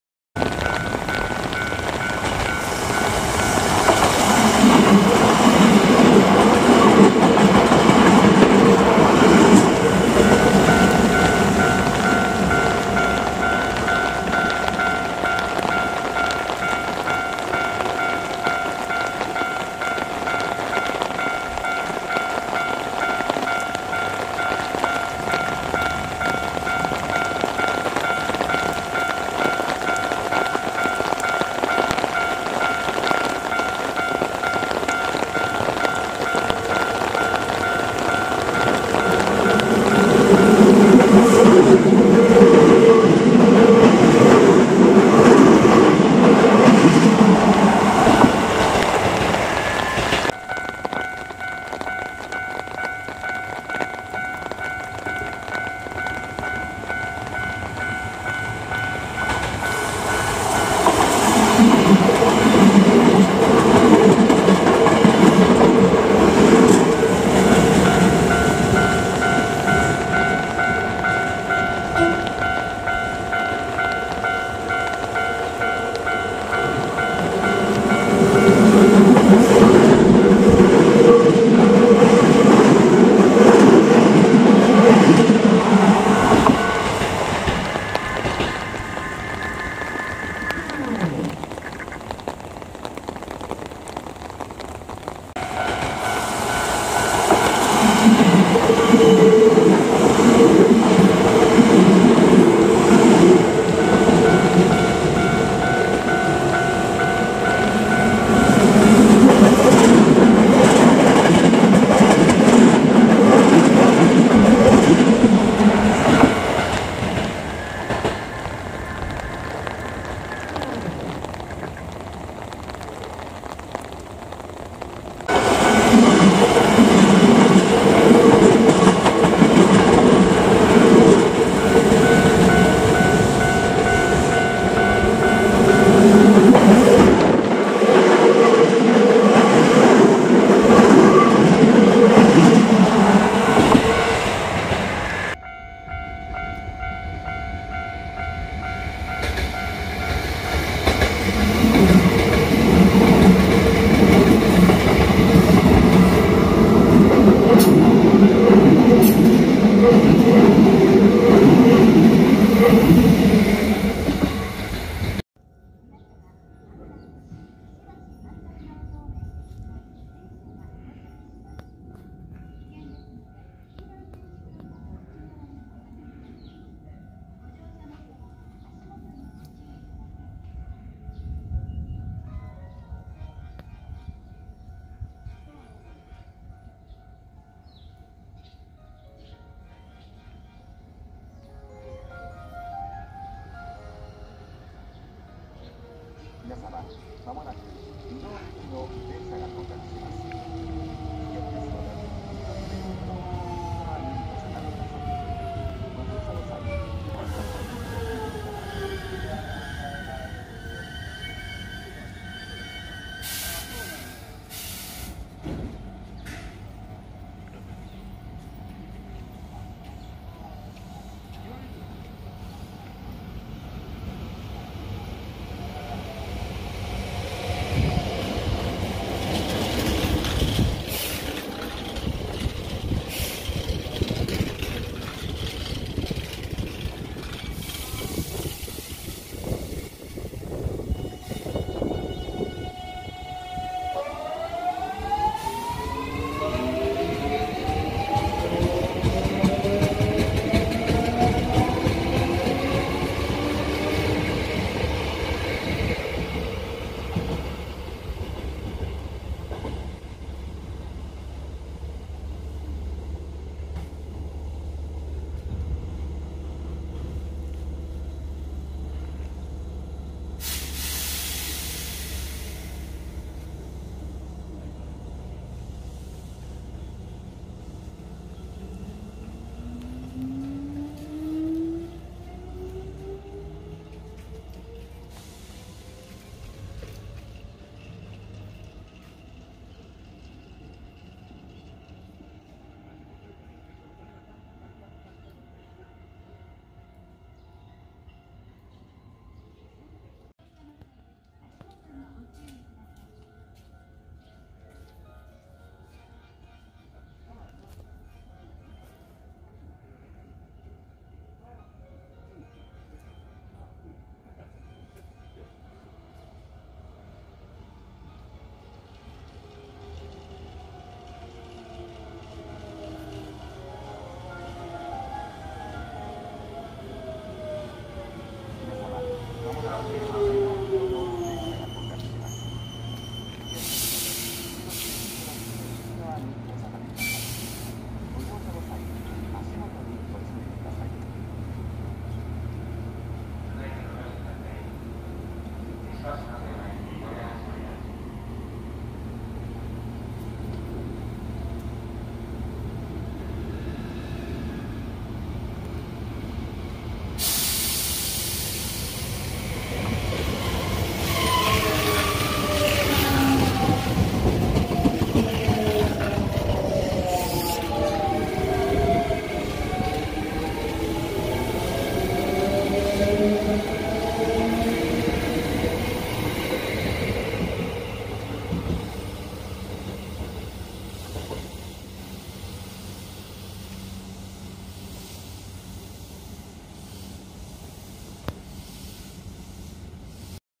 【かつての増結車天国】阪急箕面線 8000系8030F×8040F×7000系7031F
2023年6月28日撮影。
この日は日中運用に8000系8030F+8034Fの4連、代走8040F+8041Fの4連が走り、増結2連×4本が大集合。さらに早朝出庫の朝夕運用に7000系7031Fも加わり、とうとう抵抗制御の5100系(6000系は2本とも抜けています)のいない、さらに元増結運用こなしていた車両ばかりの箕面線となりました。